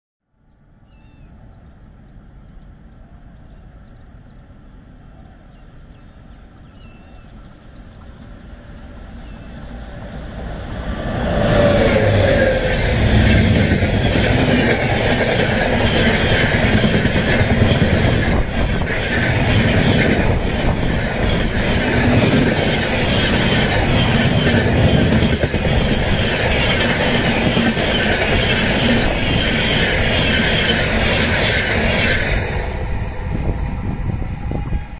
cinematic video | AVANTIKA express skipping Vikramnagar in speed
the train in the video is avantika express
DDR /Dadar Western -- EKNR /Ekta Nagar (Kevadiya) plus INDB/Indore Junction -- MMCT/Mumbai Central
this train is a unique train and is a example of a rake sharing train . Rake sharing is adoptedi in Indian Railways for improvement of the performance of every rake. It increases the train numbers capacity originating from a certain station. It decreases the time of rake placement at the platform. Rake sharing is perfect using of one rake for it's connecting train numbers.